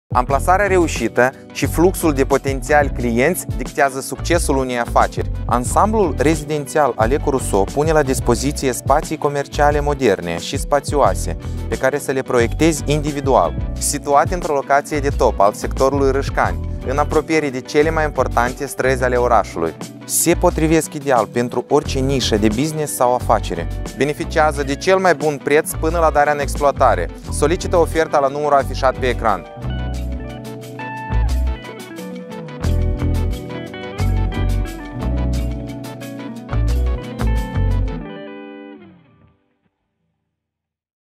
Spații comerciale în ansamblul Alecu Russo
Vrei să investești strategic într-un spațiu comercial care îți aduce venit activ? Ansamblul Alecu Russo concentrează oportunități demne pentru afacerea ta de succes.
✔️ Amplasarea strategică, la numai 10 minute de centru și 250 m de principalul bulevard al sectorului Râșcani.
✔️ Flux continuu de potențiali clienți.
✔️ Infrastructură de transport convenabilă și conexiune cu arterele principale ale orașului.
✔️ Spații comerciale moderne, pe care să le proiectezi individual, în funcție de nișa de business pe care o deții.

Beneficiază de cel mai bun preț, până la darea în exploatare.